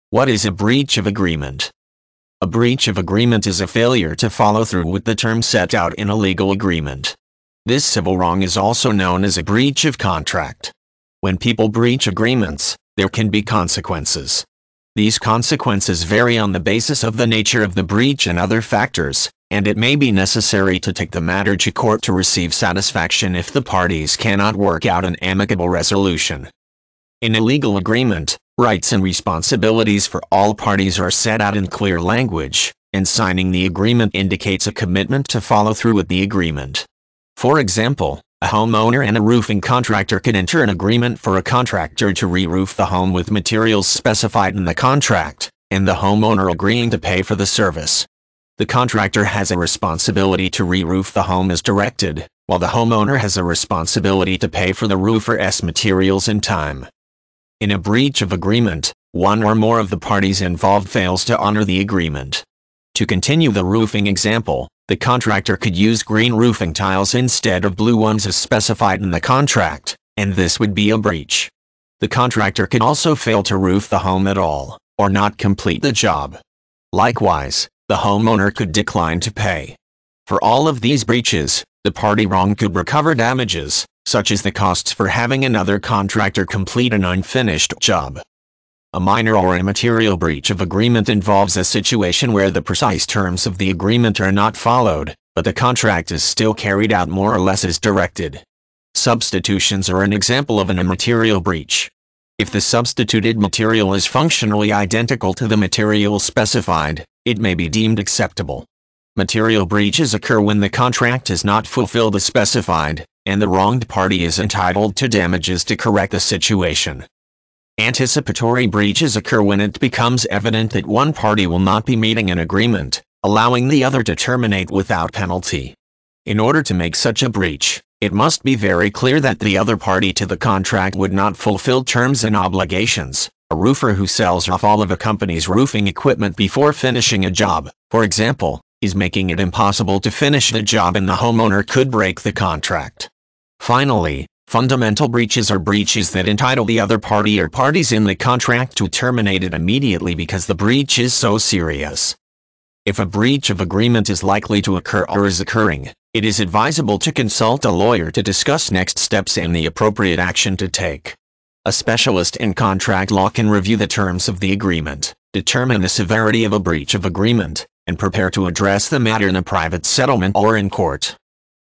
What is a Breach of Agreement?
A breach of agreement is a failure to follow through with the terms set out in a legal agreement. This civil wrong is also known as a breach of contract. When people breach agreements, there can be consequences. These consequences vary on the basis of the nature of the breach and other factors, and it may be necessary to take the matter to court to receive satisfaction if the parties cannot work out an amicable resolution.